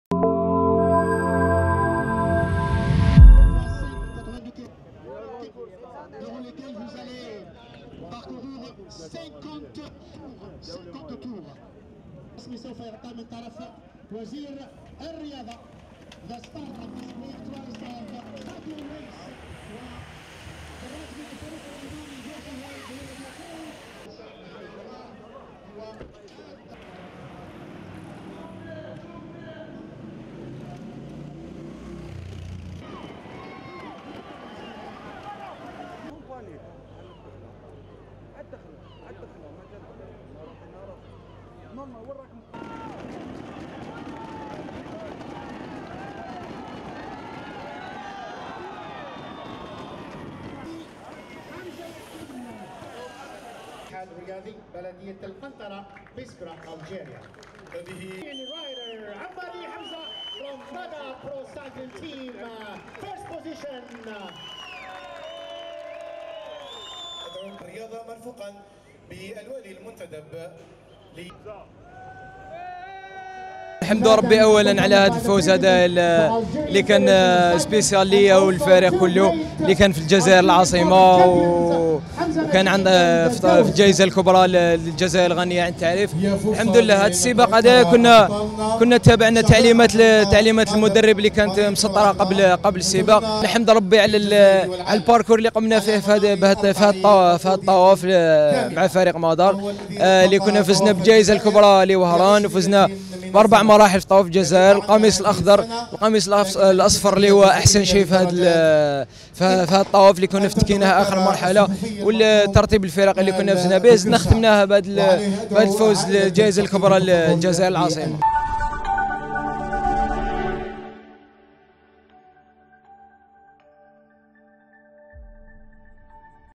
الجائزة الكبرى لمدينة الجزائر للدراجات : فوز حمزة عماري بالسباق
جائزة الجزائر الكبرى للدراجات